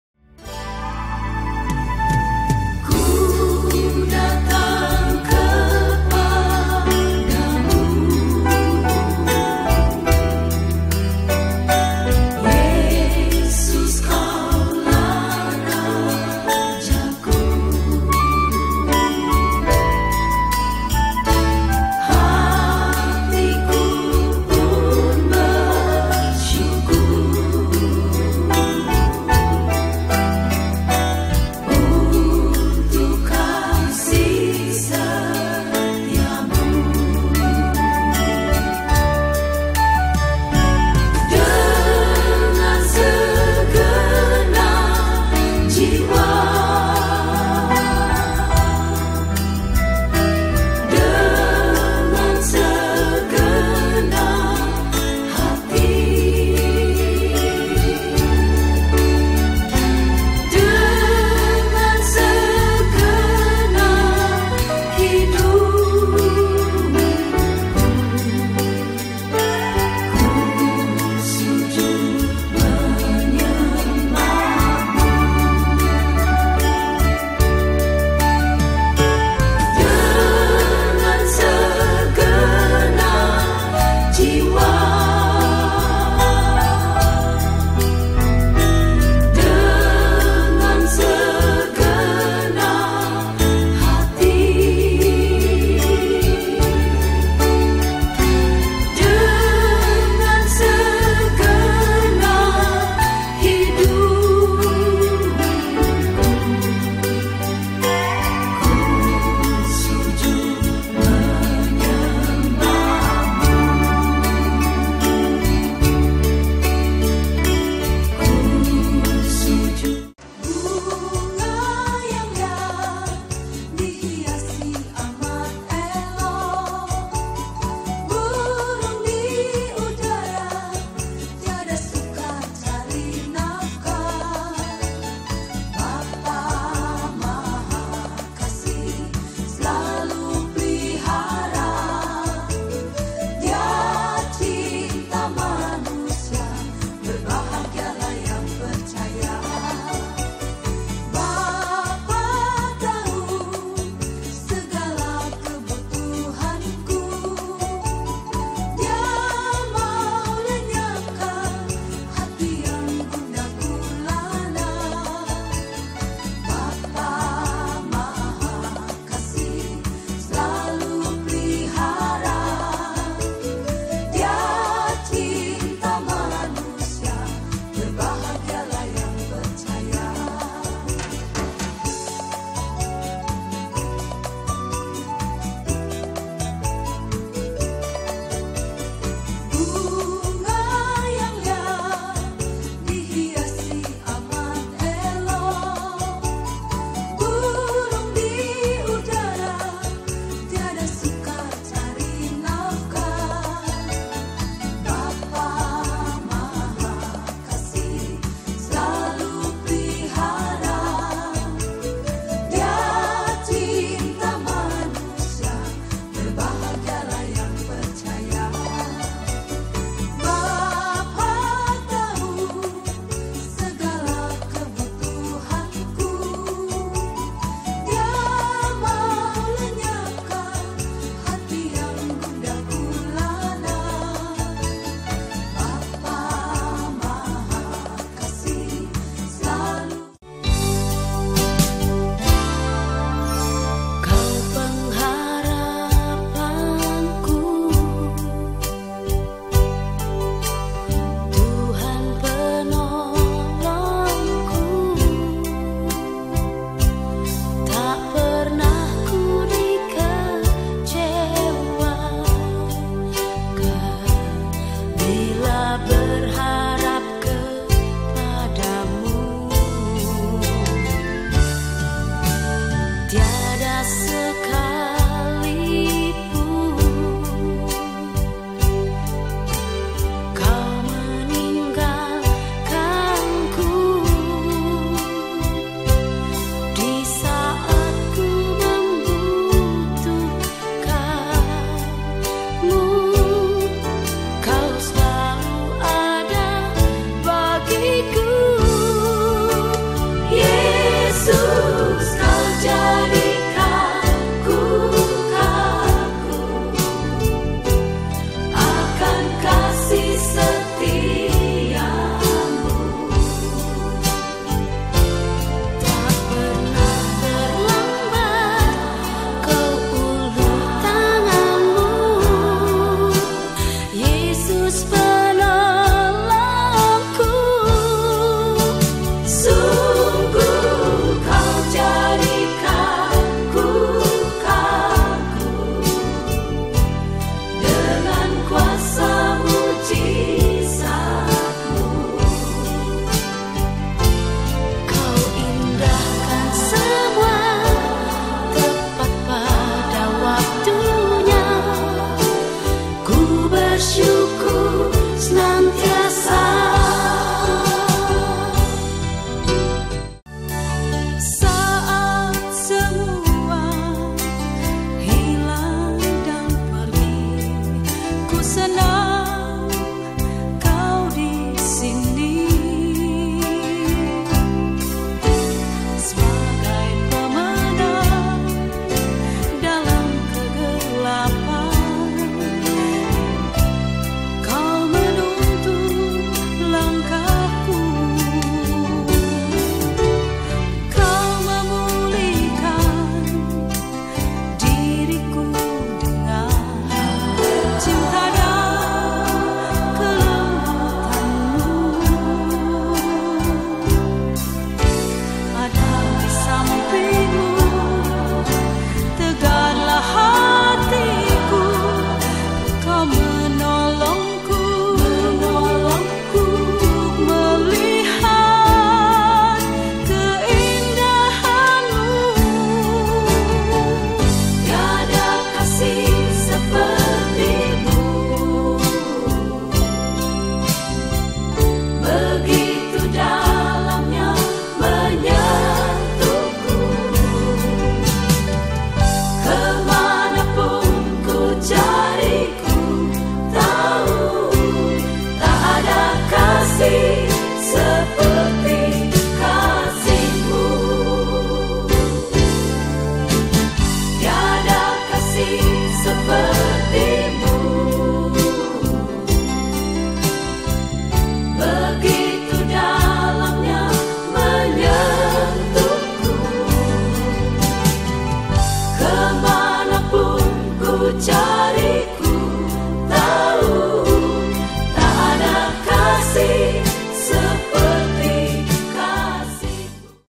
Walking around Hongkong Island and Kowloon Hongkong @2020😍
Enjoy my day off on Sunday and spend time to walking around in the city of Hongkong and Kowloon city in Hongkong @2020 😍♥️😍♥️